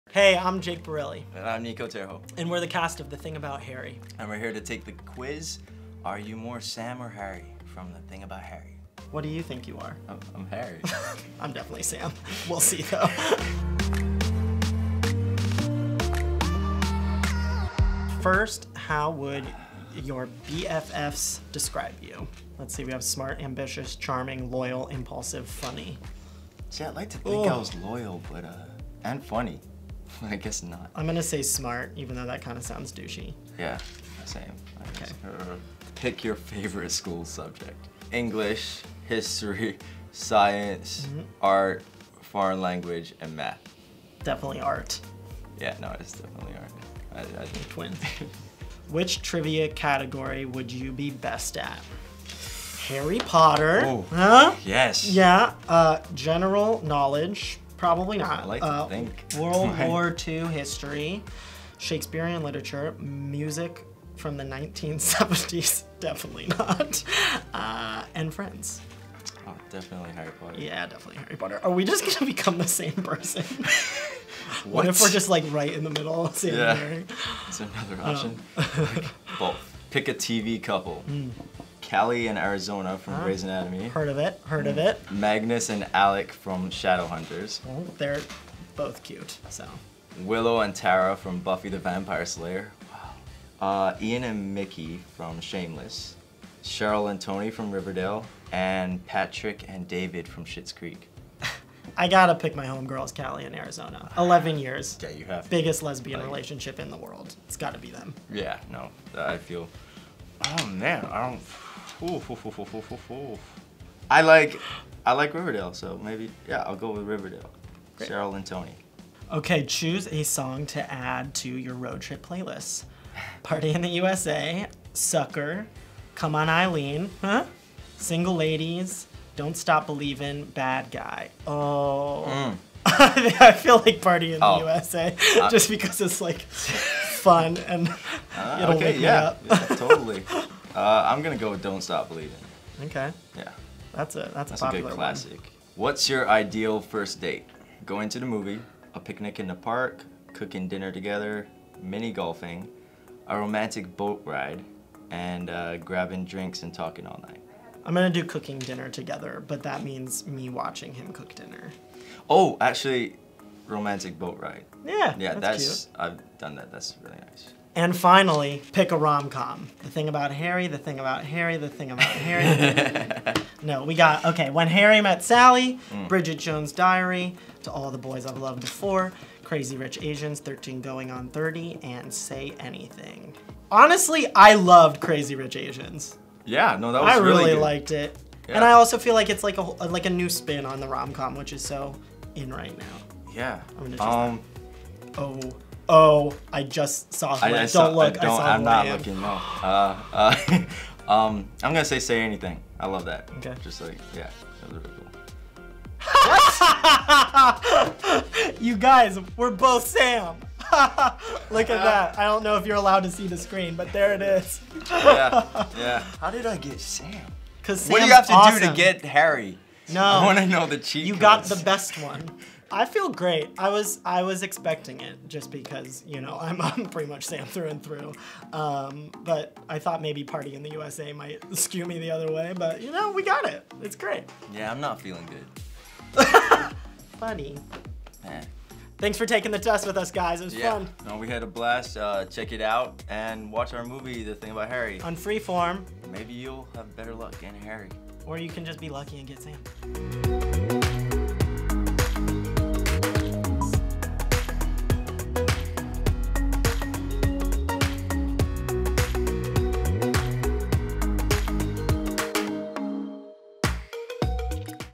"The Thing About Harry" Cast Finds Out Which Characters They Really Are
Jake Borelli (Grey's Anatomy) and Niko Terho take our quiz to see if their personalities match their characters!

BuzzFeed Celeb
MUSIC